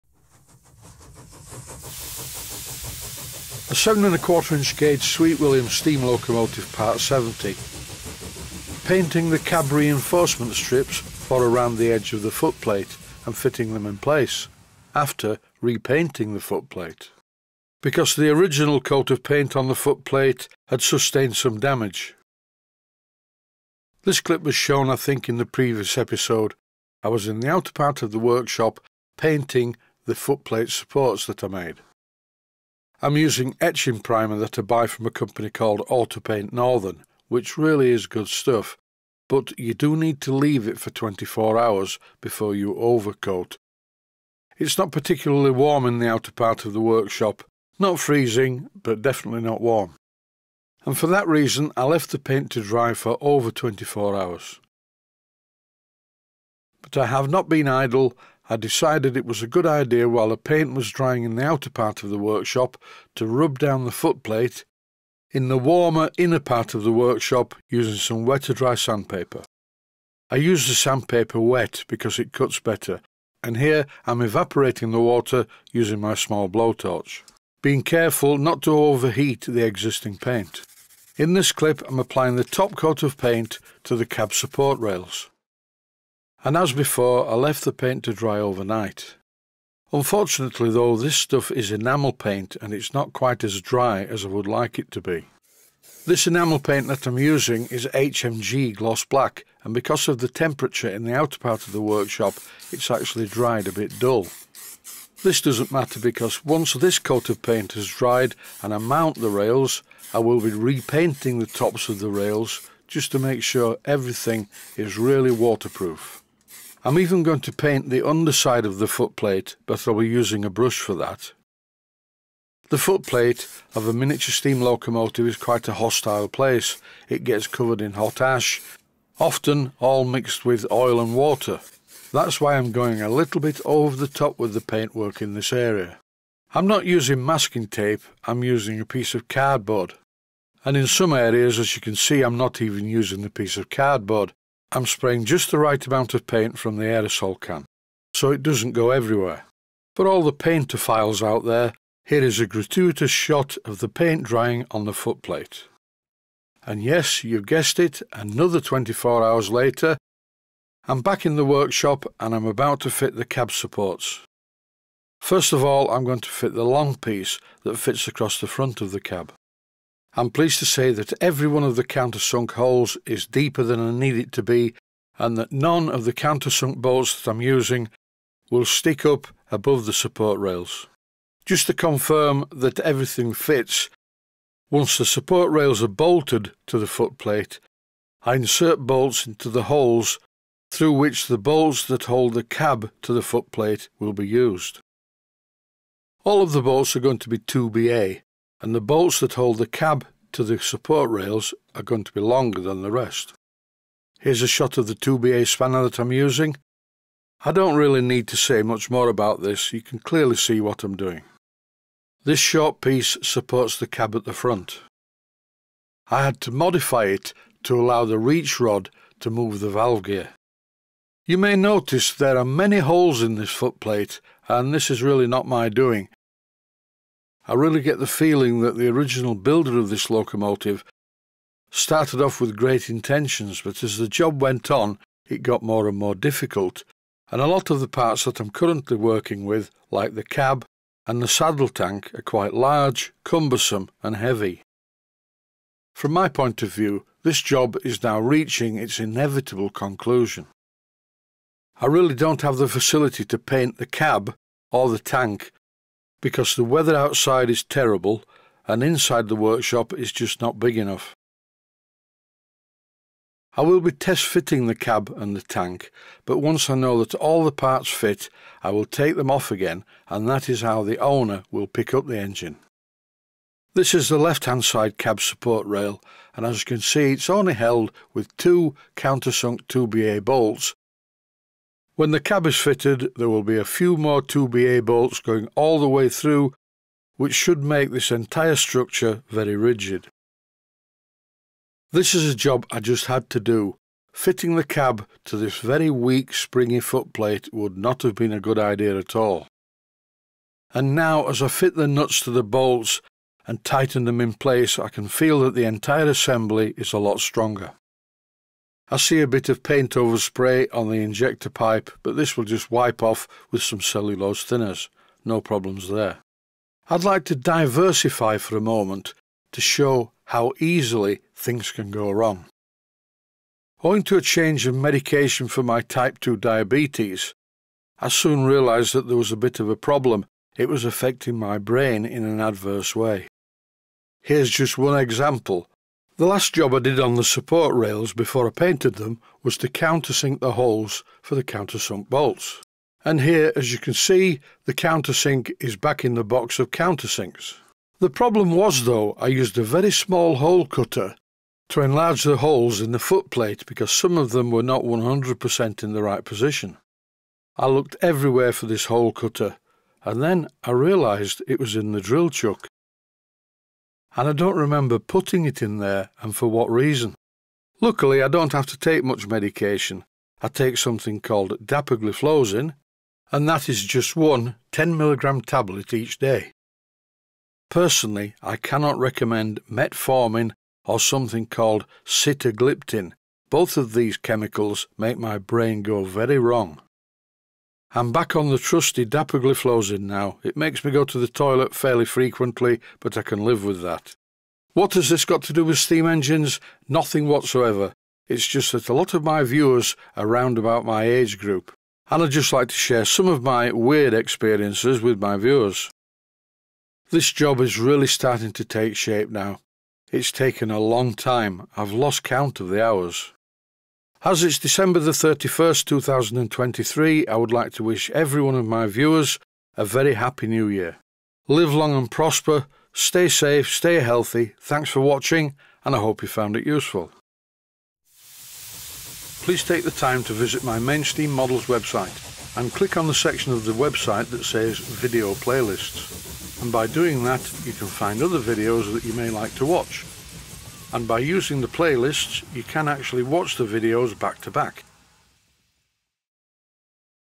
A 7 ¼ INCH GAUGE "SWEET WILLIAM" LOCOMOTIVE - PART #70
A 7 ¼ Inch Gauge "Sweet William" Locomotive - Part #70. Painting the Cab Reinforcement Strips for around the edge of the Footplate and fitting them in place after repainting the Footplate.